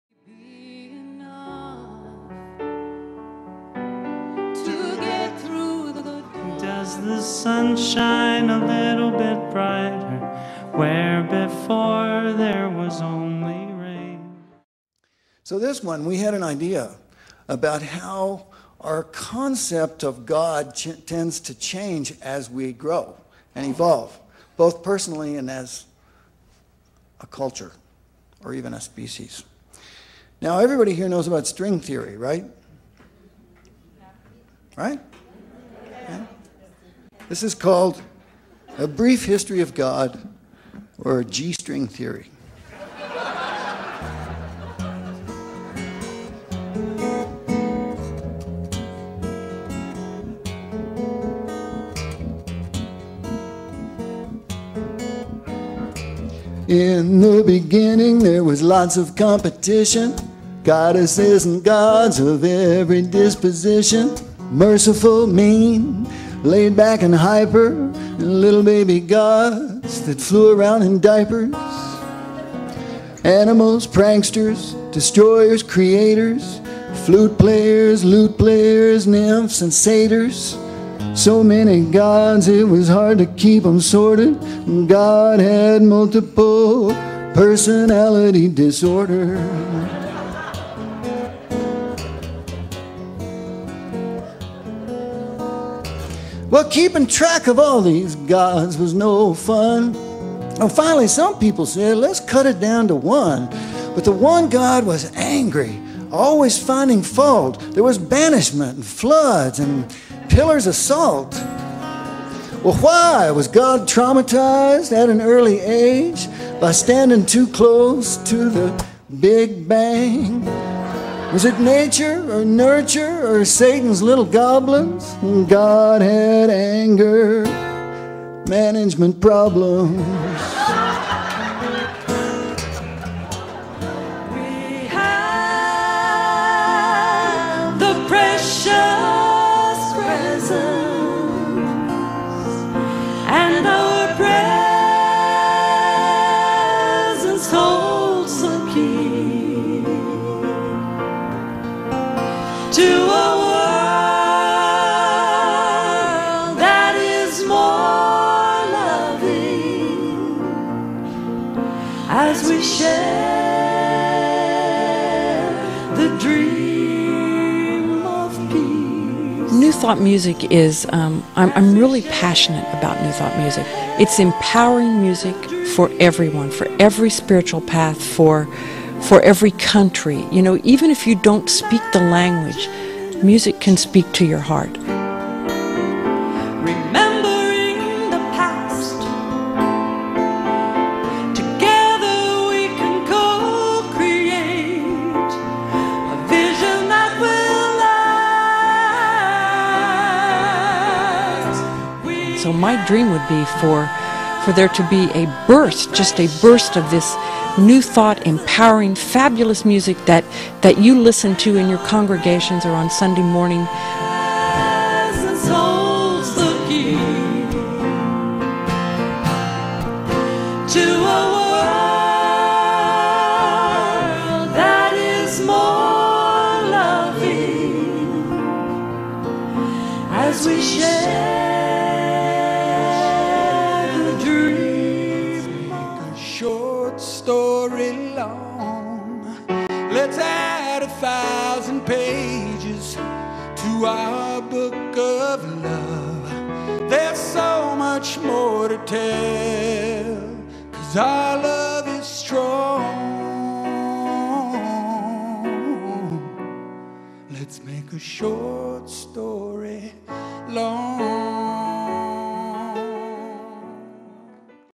2011 PosiPalooza preview
PosiPalooza will be here soon! Coming to the Historic Paramount Theatre, Abilene, Texas on Friday, October 28th. $20 at the door, so get your tickets early, presale is $15 and for students, military, police, and seniors (if you say your are, we will believe you) are $10 and 12 and under are free. So bring your friends and family for a wonderful, uplifting, positive experience!